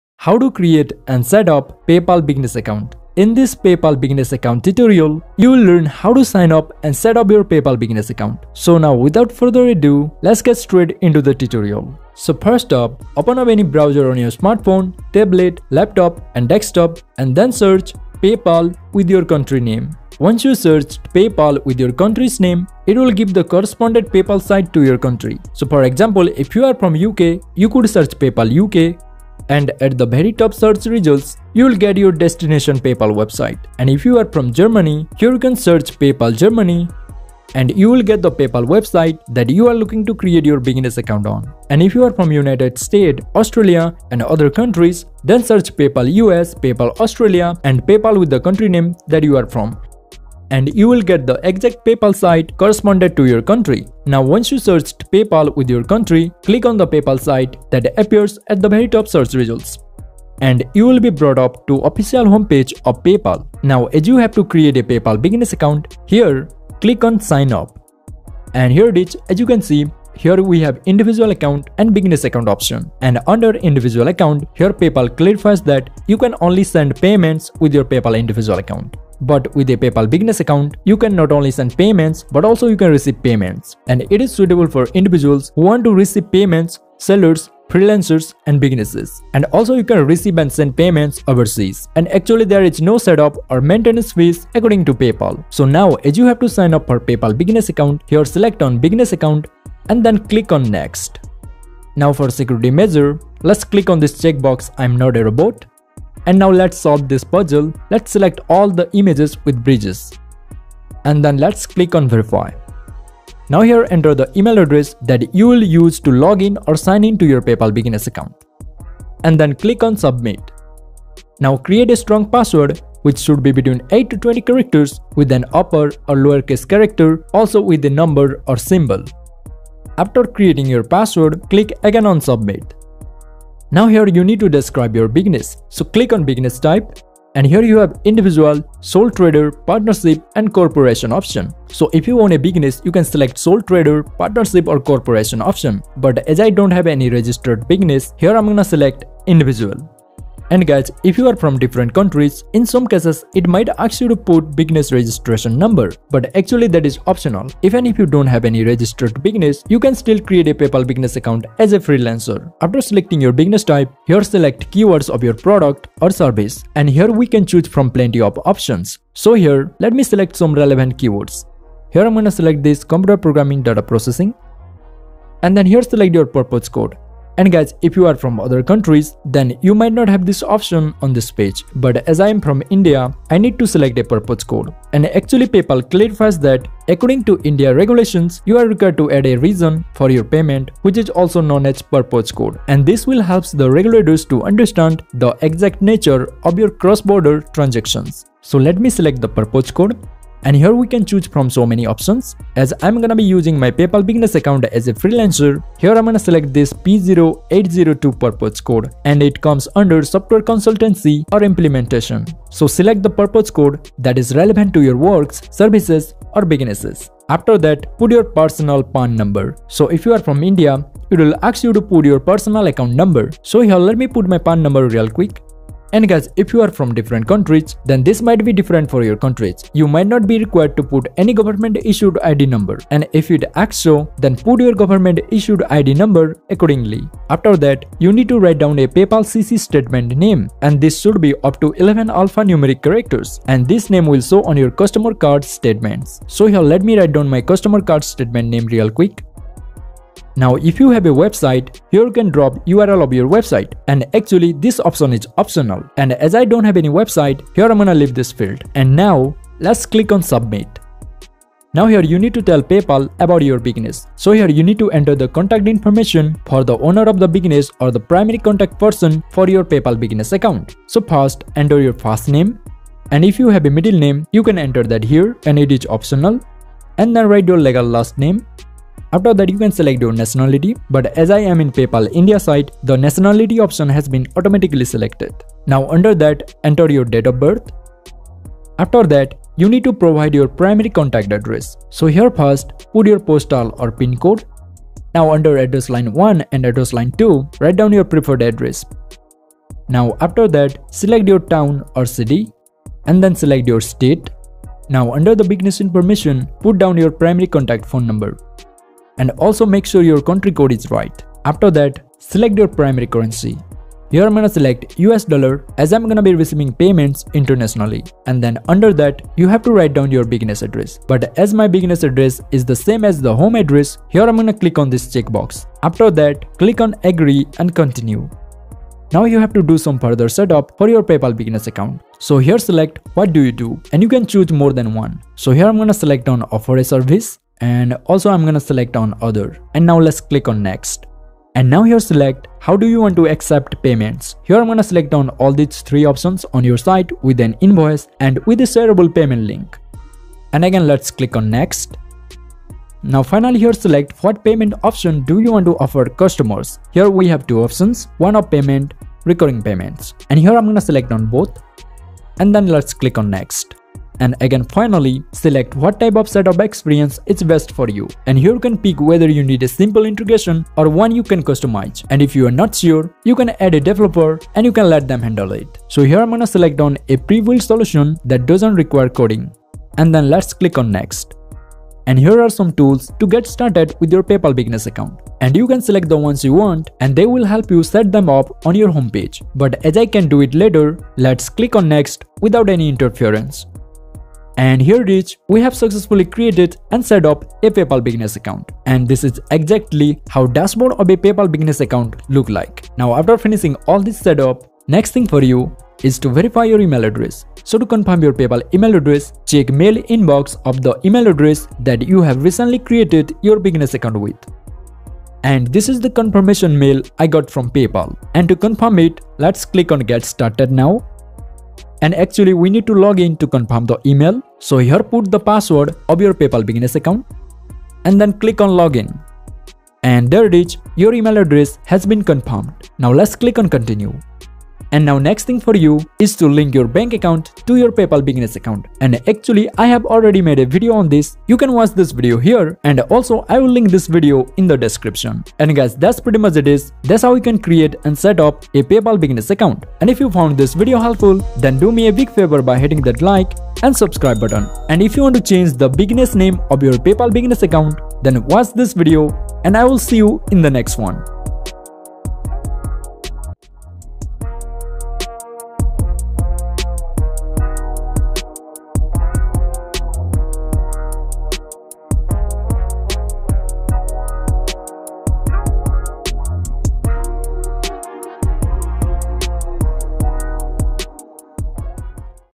How to Create & Set Up PayPal Business Account | Paypal Business Account Tutorial
How to create and set up a Paypal business account step by step.

In this video, you'll learn how to make a Paypal business account as well as how to set up that paypal business account step-by-step from start to end. So if you're planning to make a paypal business account so that you can send as well as receive payments international with ease, then this video is going to be very helpful for you and you can easily create and set up your business account on Paypal.

This video will cover how to sign up for Paypal business account and then how to set up that paypal business account and at the end how to verify or confirm email address on Paypal business account. After that you need to link and confirm your bank account to finish complete setup of your business account so that can start receiving payments. And that part hasn't included in this video coz I have made a separate dedicated video on how to link bank to paypal and you can watch that video